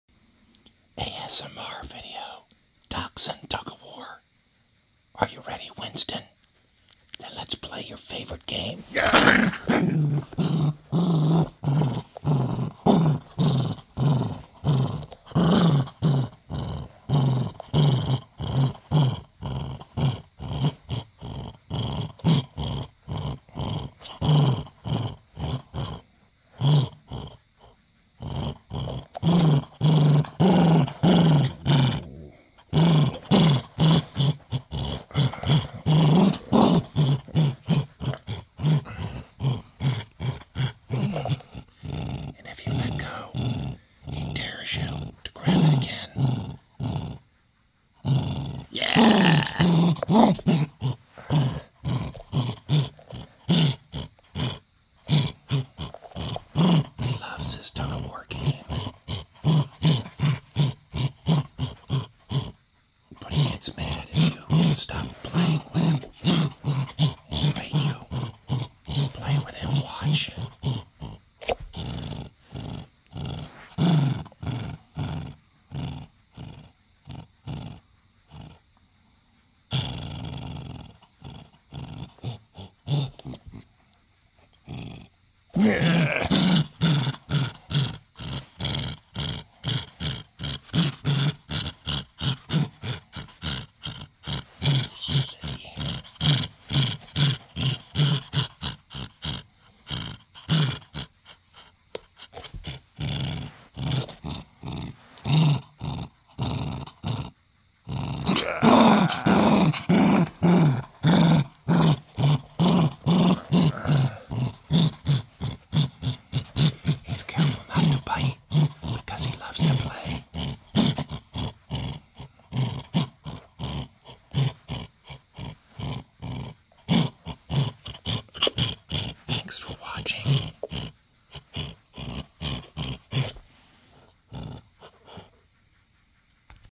ASMR Dachshund Tug A War!
This is NOT a train related video . As marked it is ASMR which is a YouTube video category for sound videos which I occasionally upload to try and expand the channel. My dog does not like giving up his many toys